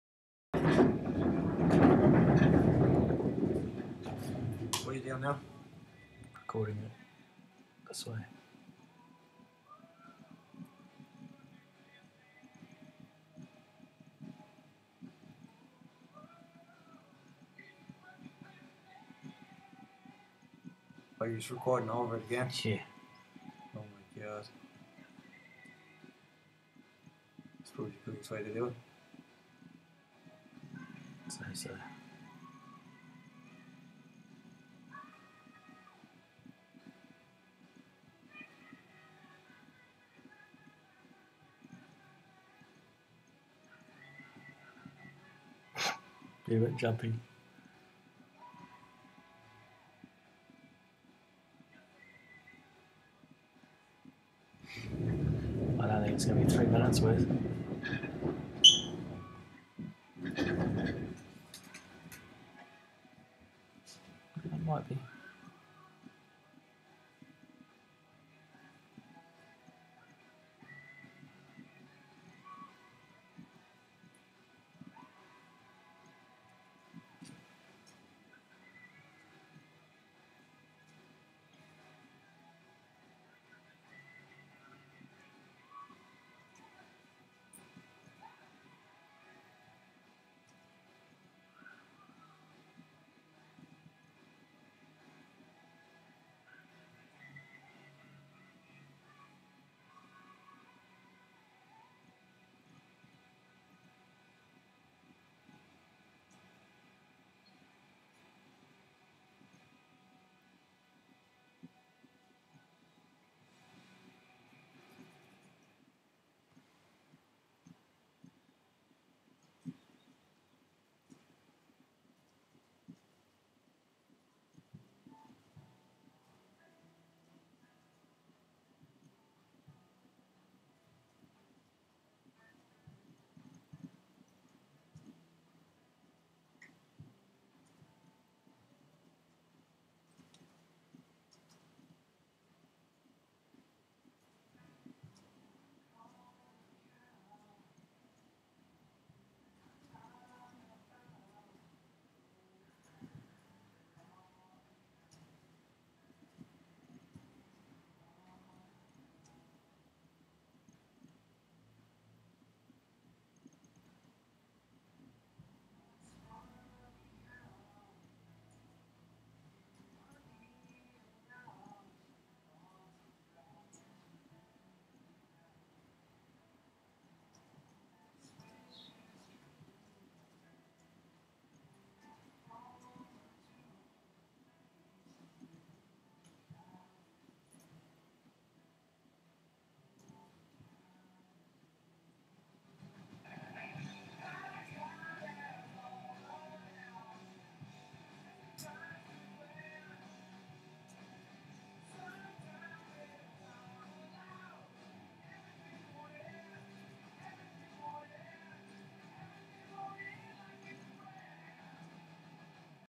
Speed Painting 27.01.14
A quick digital painting/Illustration - Real time is 1hr, Original file was huge so the editing is average lol.